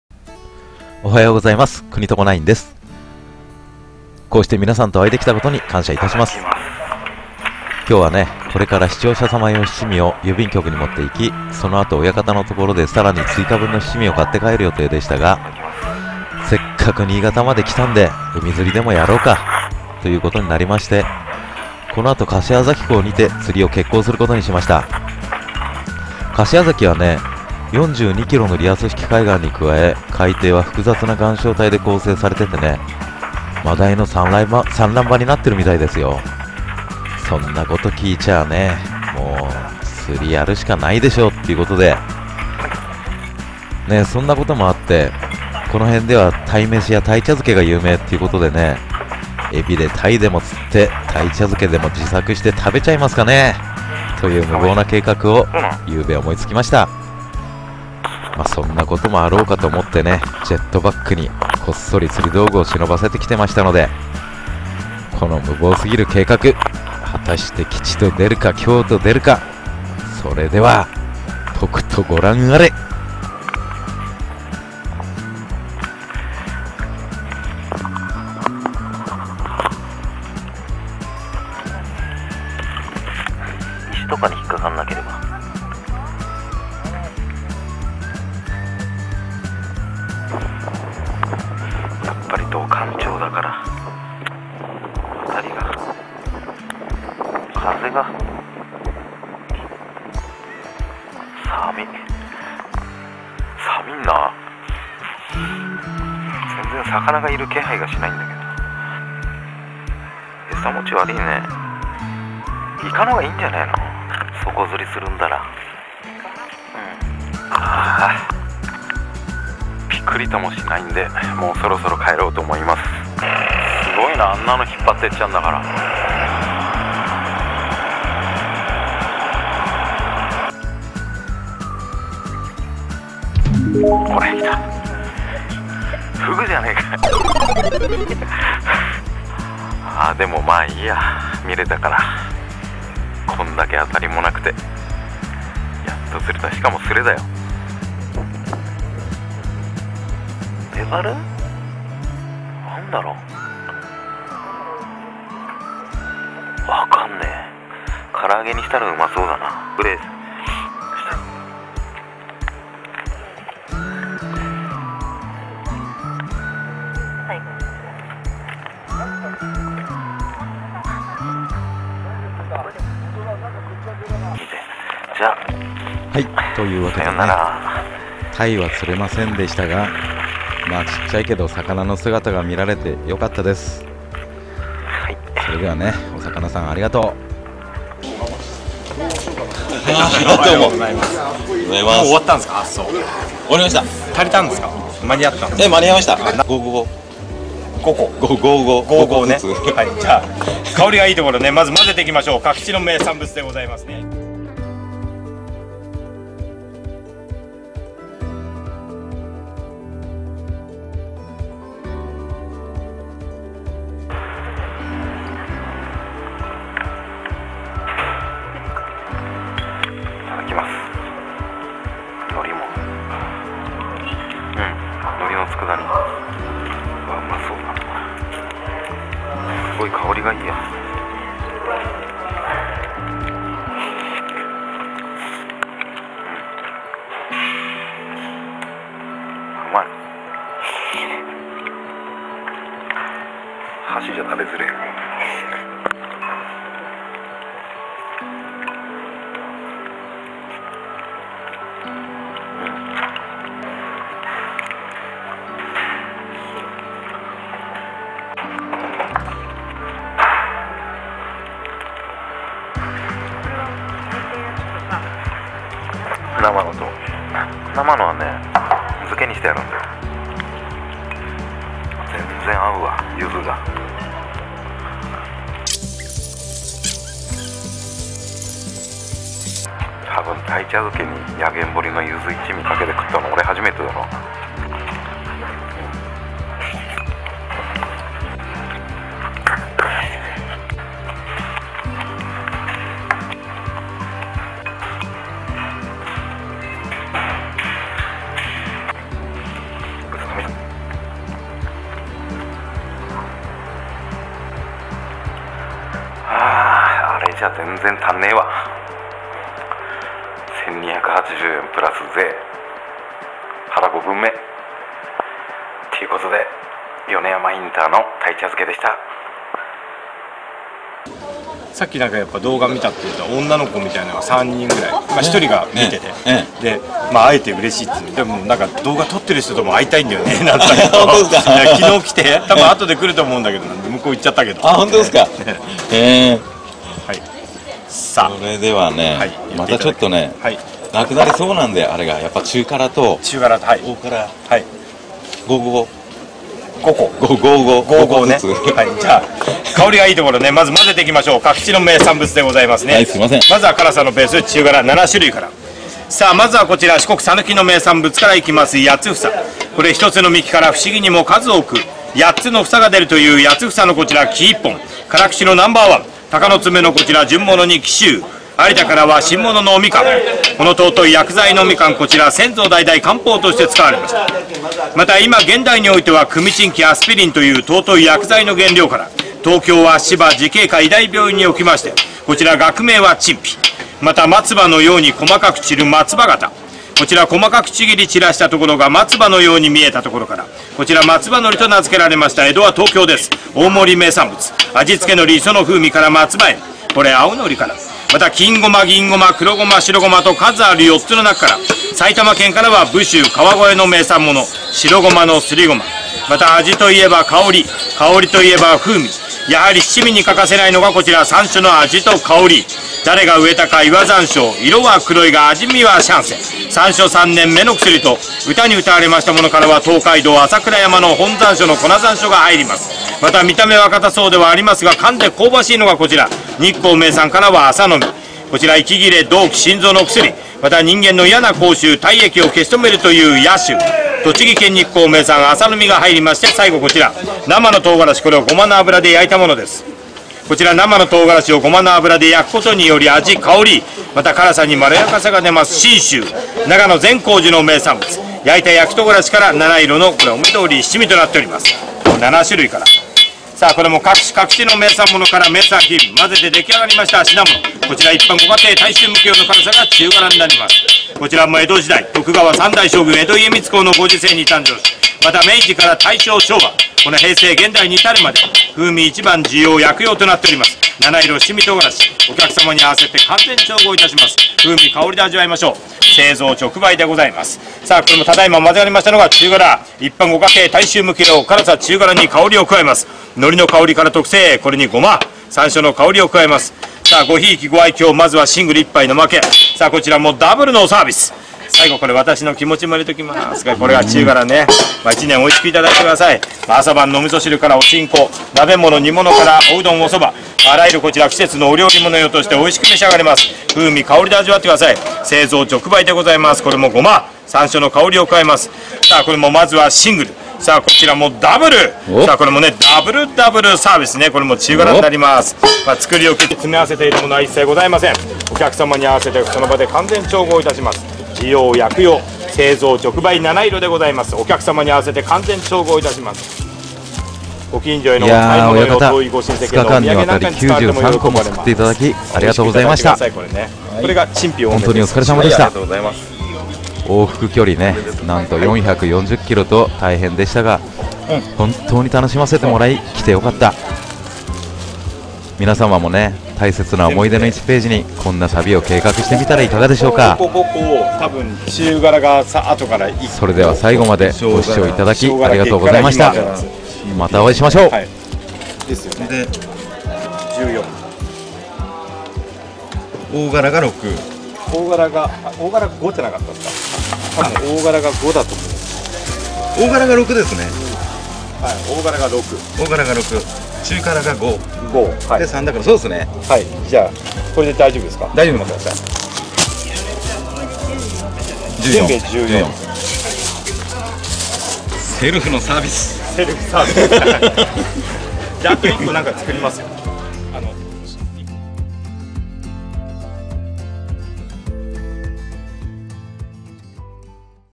親方七味を持って旅に出る番外編柏崎港で鯛釣って
みなさんこんにちは！kunitoko9です、
今日はこれから視聴者様用
七味を郵便局に持って行き
その後、親方のところで
更に追加分の七味を
買って帰る予定でしたが、
せっかく新潟まで来たので
海釣りでもやろう！
という事になりまして
このあと柏崎港にて
釣りを決行する事に、、
この辺では「鯛茶漬け」
が有名ということで
海老で鯛でも釣って
「鯛茶漬け」でも
自作して食べちゃいますかね～
という無謀な計画を
なんと夕べ、企てました、、
そんな事もあろうかと
ジェットバックに
こっそり釣り道具を
忍ばせて来ていましたので、、
この無謀過ぎる計画、、
果たして吉と出るか
凶とでるか、、
それではとくとご覧あれ！！